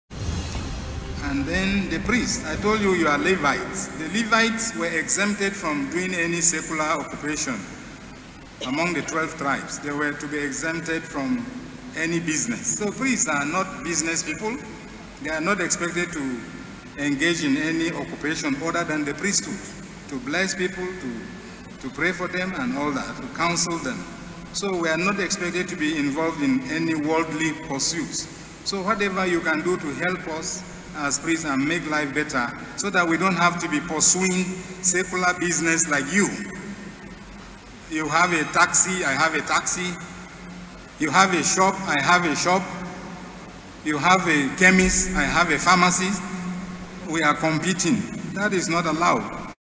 ARE PRIEST ALLOWED TO DO BUSINESS?
ARCHBISHOP KAIGAMA SPEAKS ON THIS ONE